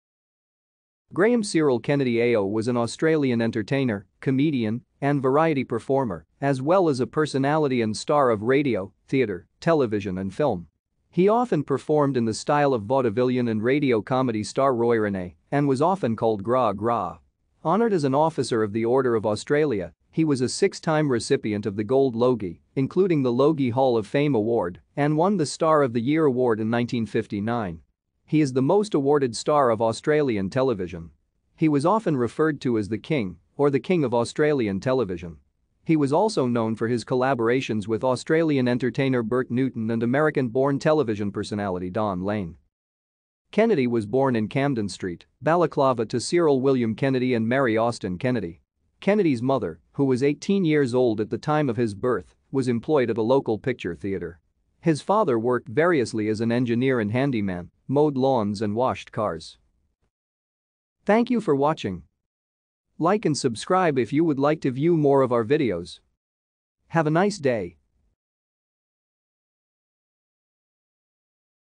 Graham Kennedy
Graham Cyril Kennedy AO  was an Australian entertainer, comedian and variety performer, as well as a personality and star of radio, theatre,  and . He often performed in the style of vaudevillian and radio comedy star Roy Rene and was often called "Gra Gra". Honoured as an officer of the Order of Australia, he was a six-time recipient of the Gold Logie, including the Logie Hall of Fame award, and won the Star of the Year Award in 1959. He is the most awarded star of Australian . He was often referred to as "The King" or the "King of Australian ". He was also known for his collaborations with Australian entertainer Bert Newton and American-born  personality Don Lane.